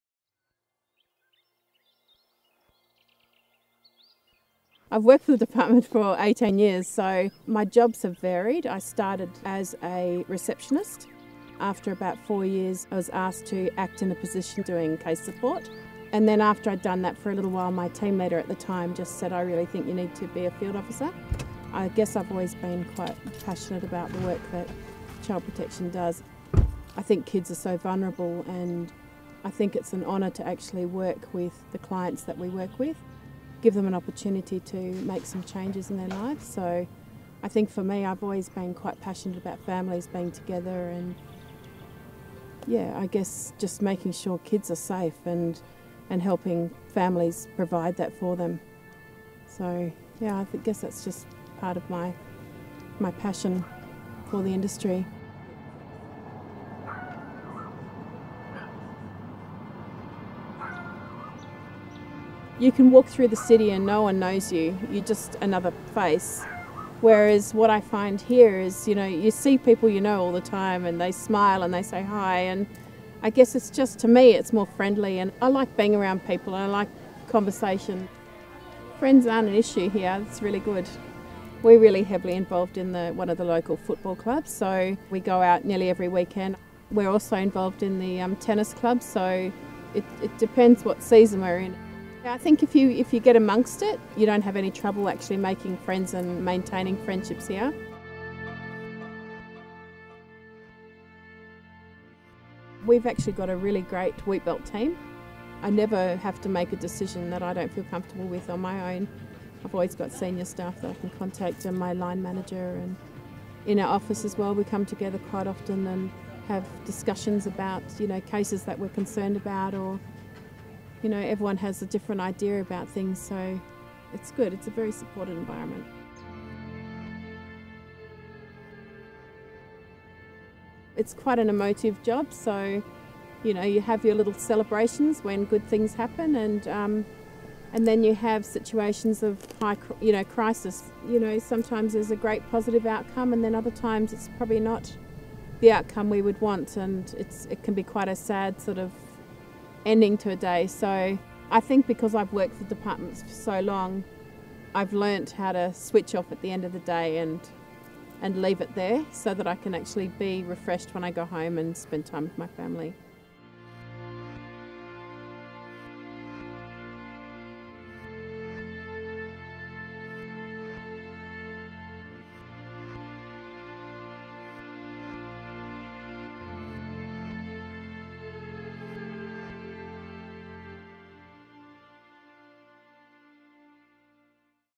A Day in the Life of a Child Protection Worker in Merredin
Hear from Philippa about what it's like to live and work in the Department for Child Protection and Family Support's Merredin district office.